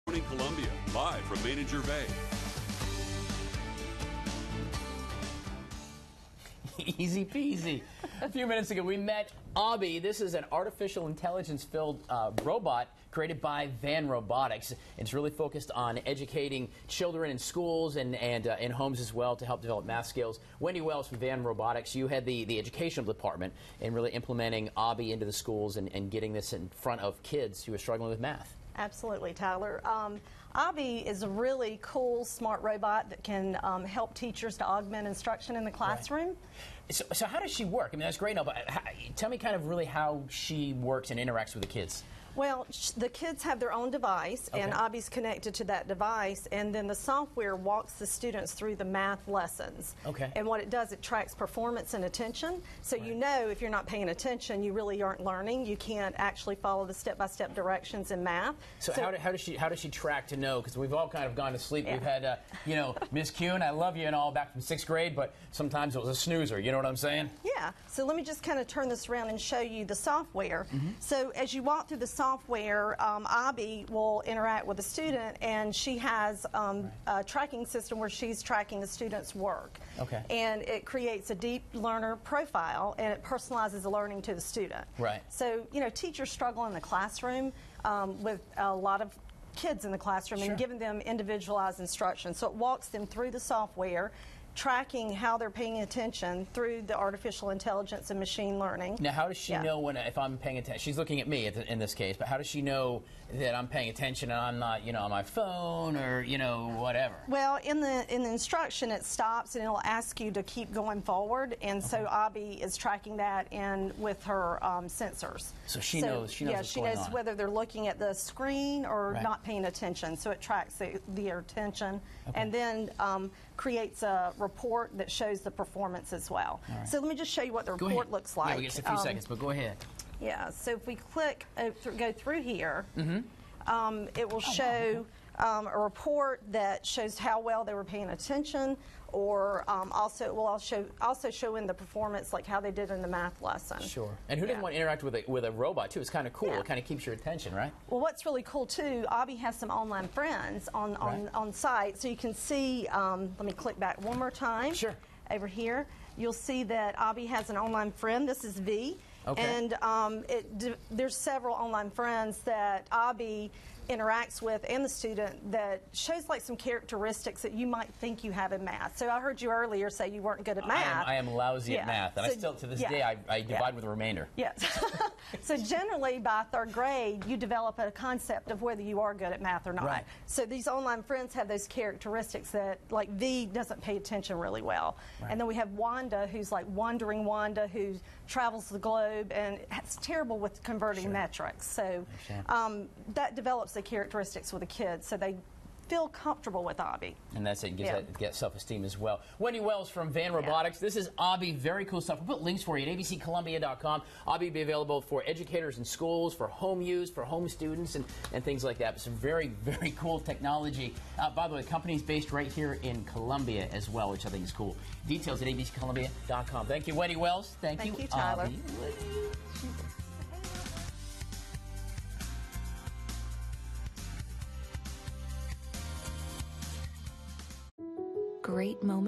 Meet ABii Part 2
ABii is an artificial intelligence robot, created by Laura Boccanfusom PhD and her team at Van Robotics, with the mission of helping students understand math.  Tyler Ryan gets an up close and personal look at this cutting edge learning tool.